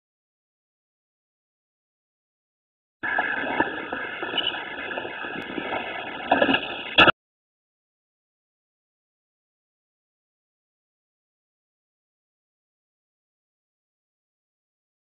Andrew ollies da 5 stair(HD)
this is a beast video of my friend andrew but we filmed this with our phones so it wouldent uplod mine because it was to big!: (